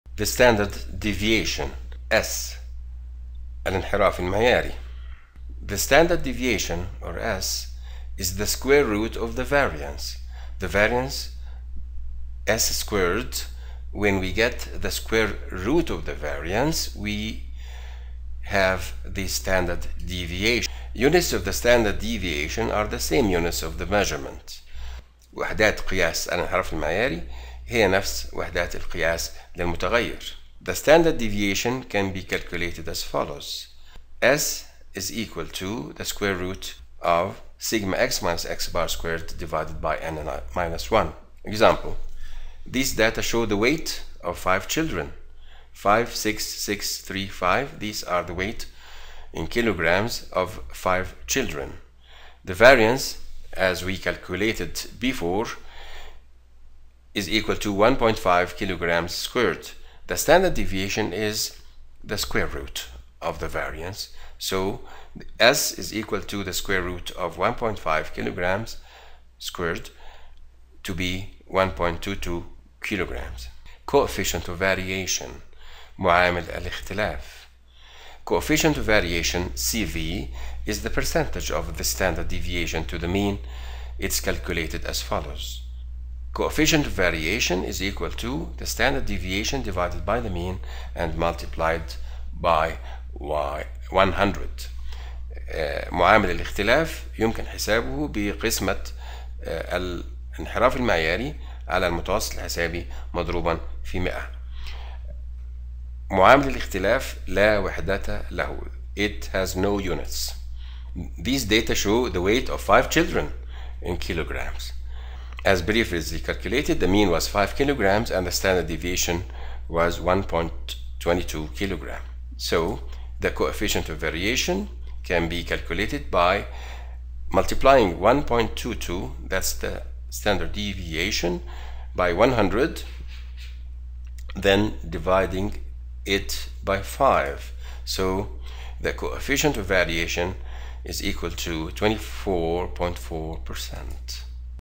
Basic Biostatistics - U1-L13 : Measures of Dispersion - 3
المقررات المفتوحة
أساسيات الإحصاء الحيوي - 141طبع Basic Biostatistics
الوحدة الأولى: الإحصاء الوصفي: First Unit: Descriptive Statistics
Measures of Dispersion
1- Standard deviation
2- Coefficient of variation